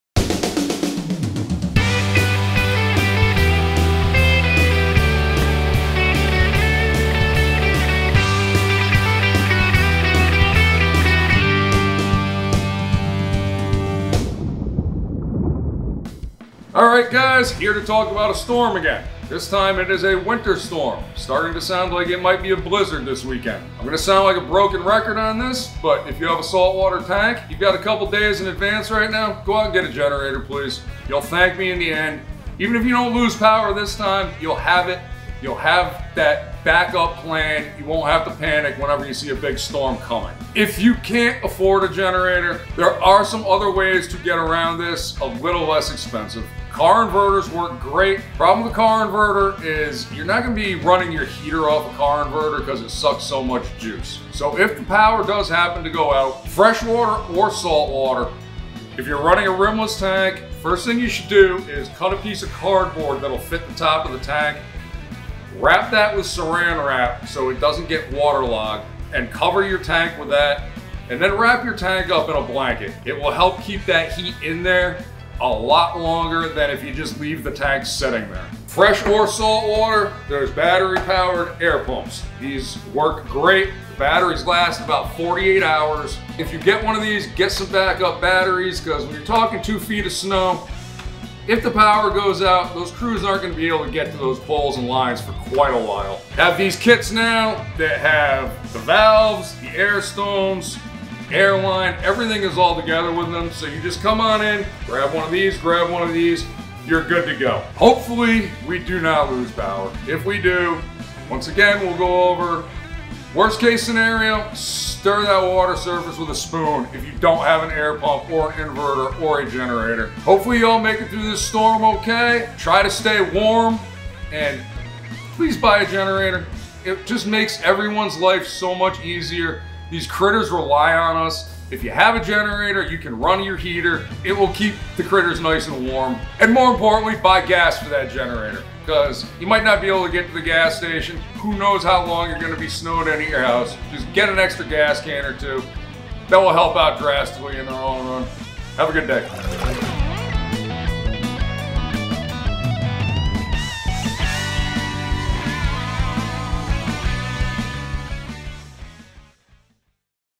How To Prepare Your Aquarium for a Blizzard
Ernie shares some helpful advice for how to keep your aquarium from failure during a potential power-outage situation. Stay warm!